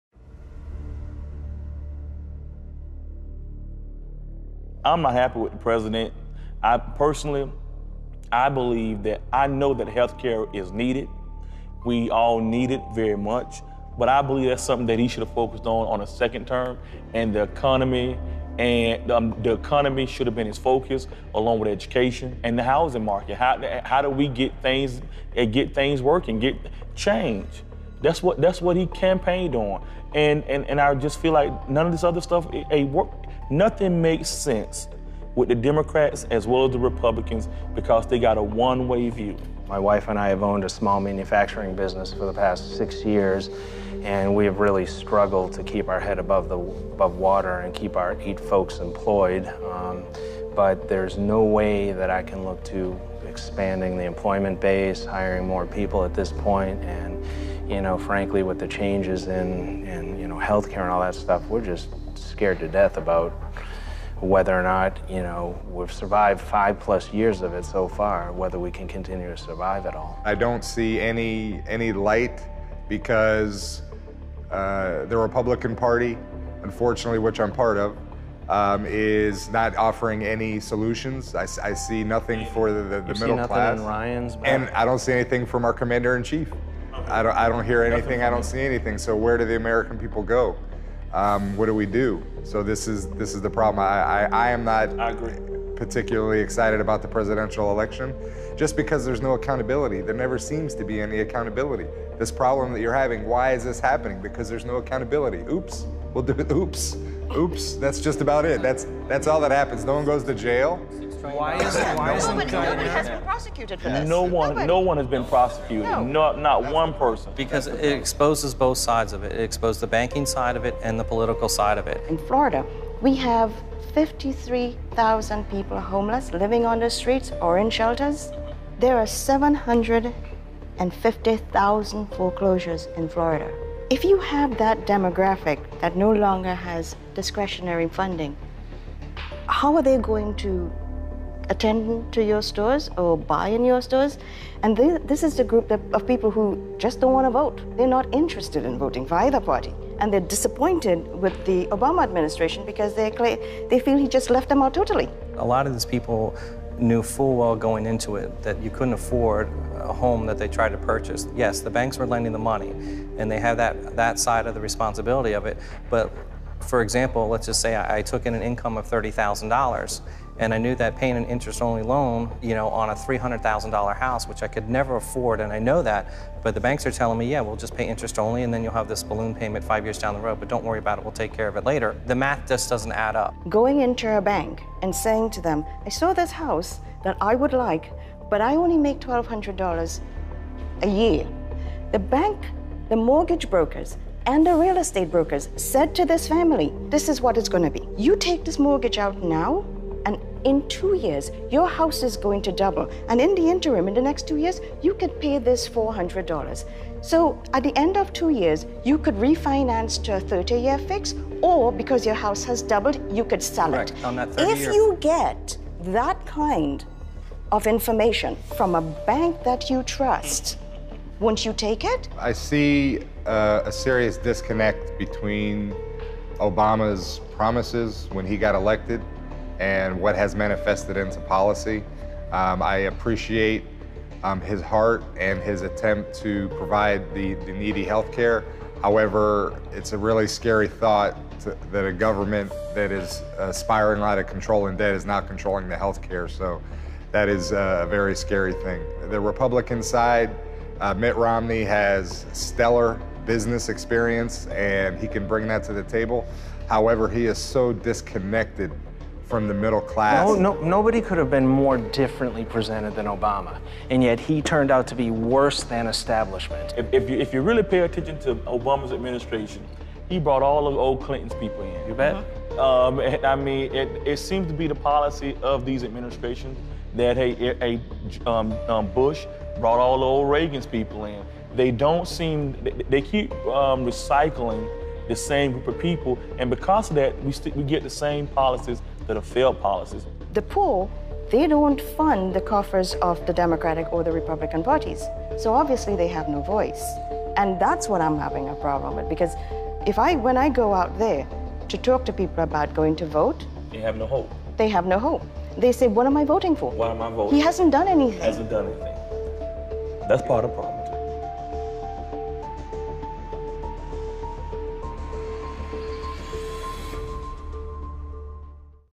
US 2012 Election Issues: Economy - Fort Lauderale
Produced by Jordan Flaherty for Al Jazeera. Featuring Bill Discipio, Susan Howai, Nezar Hamze, Vicente Robinson, and Matt Forsythe.